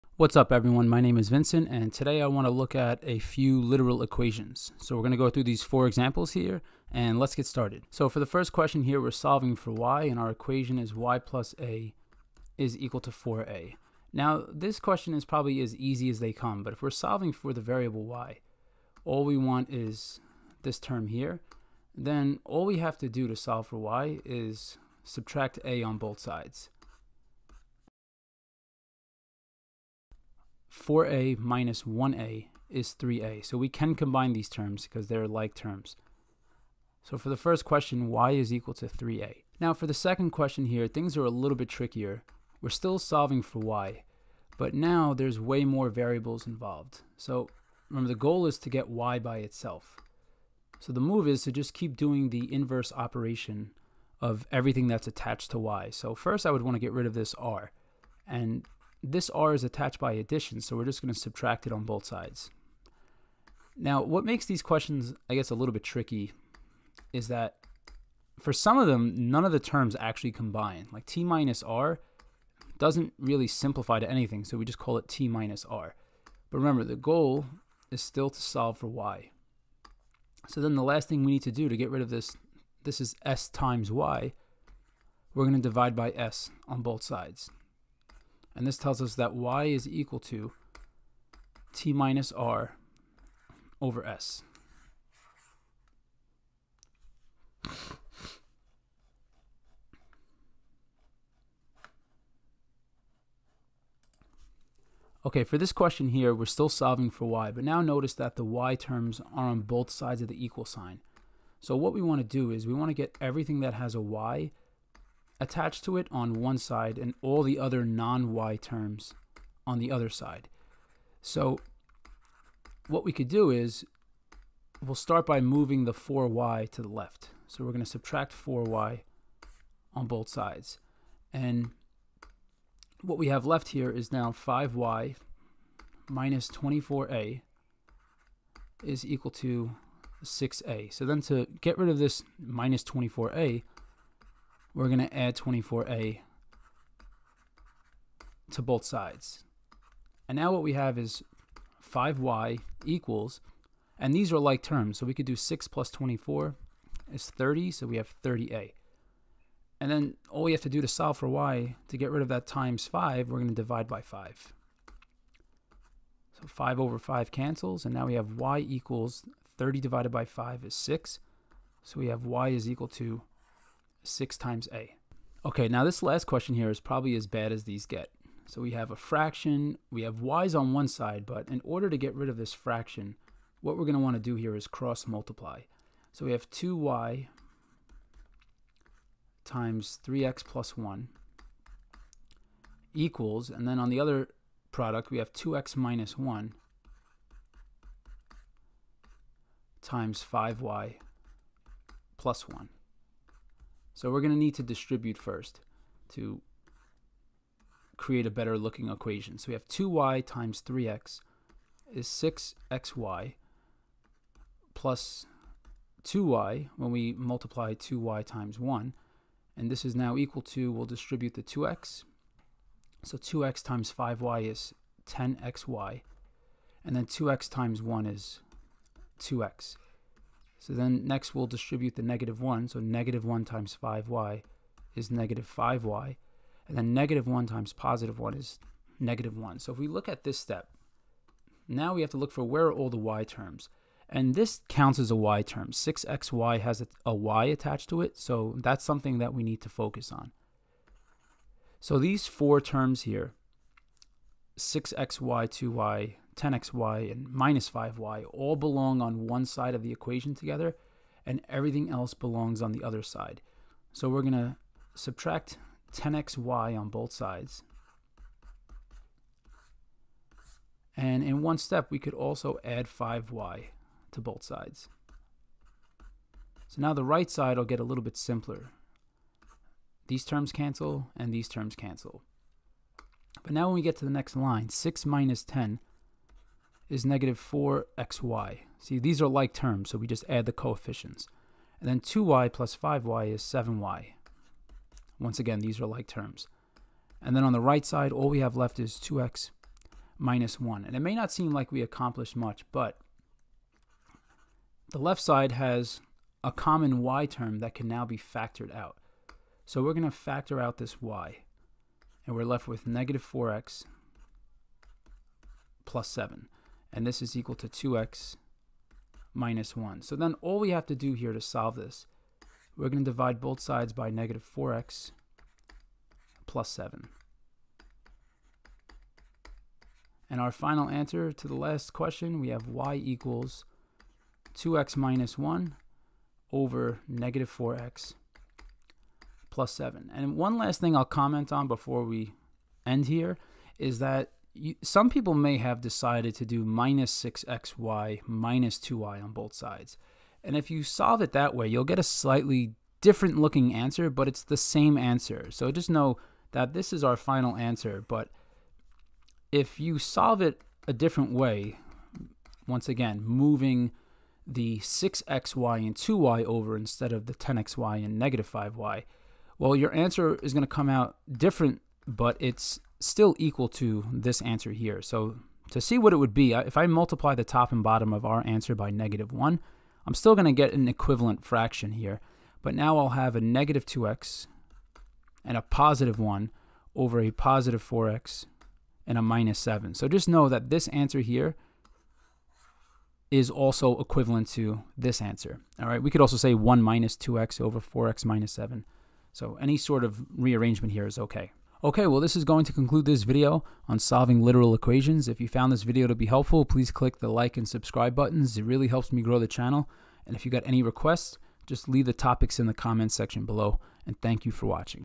Solving Literal Equations - Algebra 1
In this video, I go through 4 examples showing how to solve literal equations. The questions start off easy then increase in difficulty. The literal equations covered in this video will be helpful for a student looking to increase their algebra skills.

If you have any requests, leave them in the COMMENT section.